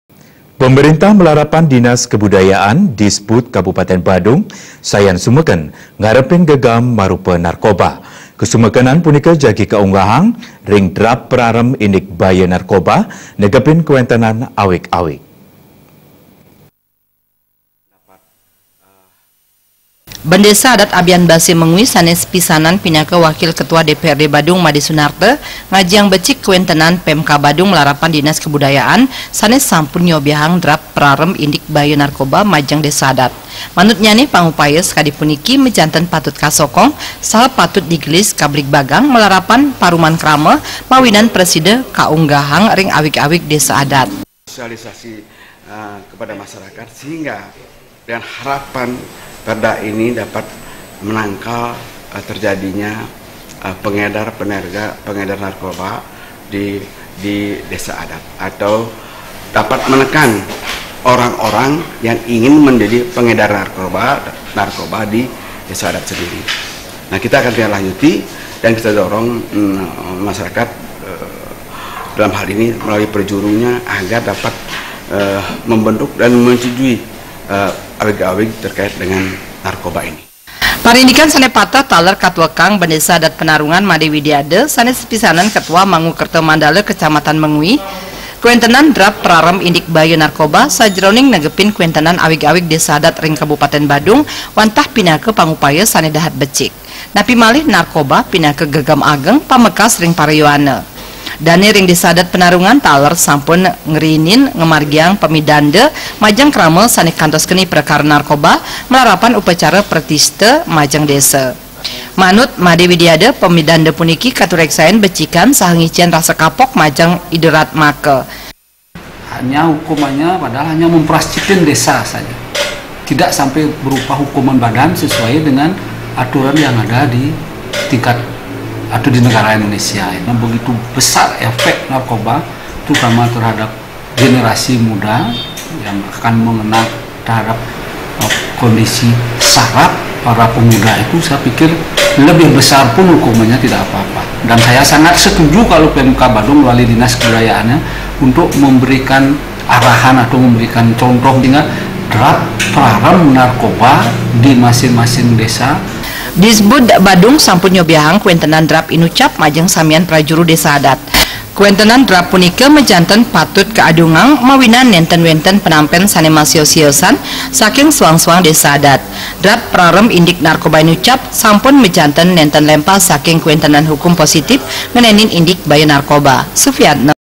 PIDABDAB PARAREM INDIK BAYA NARKOBA
PEMERINTAH MELARAPAN DINAS KEBUDAYAAN, DISBUD, KABUPATEN BADUNG, SAYAN SUMEKEN NGAREPIN GAGEEM MARUPA NARKOBA. KASUMEKENAN PUNIKA JAGI KAUNGGAHANG RING DRAFT PARAREM INDIK BAYA NARKOBA, NEGEPIN KAWENTENAN AWIG-AWIG.